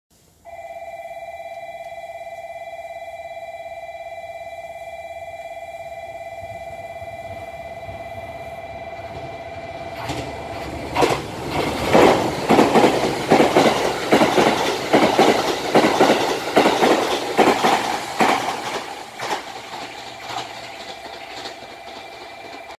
(1145)JR長崎本線 多良駅にて、787系 博多行き特急かもめ14号の撮影
長崎9:48発→博多11:53着

停車駅…長崎・浦上・諫早・肥前鹿島・佐賀・新鳥栖・鳥栖・二日市・博多

肥前山口には停車しません

毎日運転